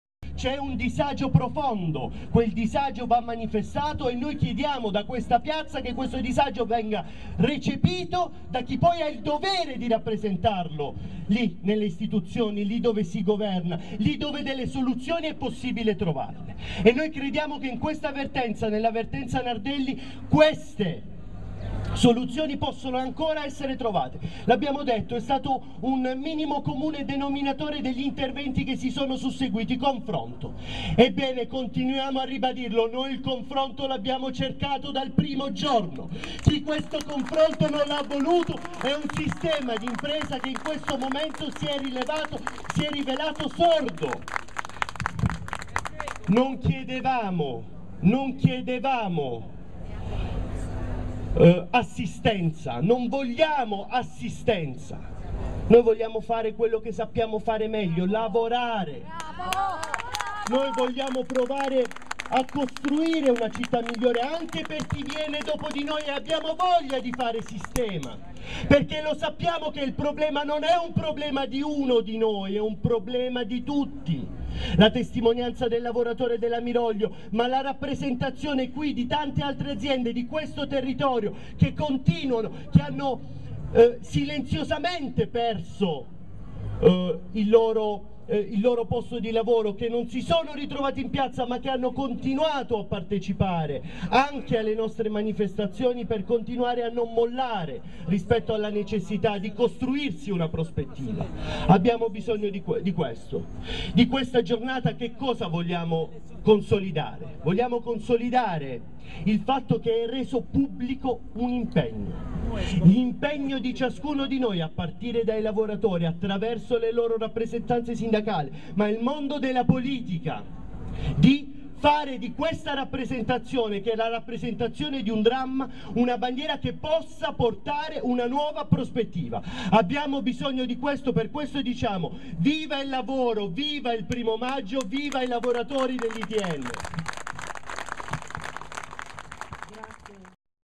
Primo Maggio 2012: Giuseppe Massafra interviene alla manifestazione dei lavoratori dell'ITN
Martina Franca: dopo l'apertura della procedura di mobilità, gli operai ITN organizzazione una manifestazione. Nel video, l'intervento di Giuseppe Massafra, segretario generale della Filctem Cgil Taranto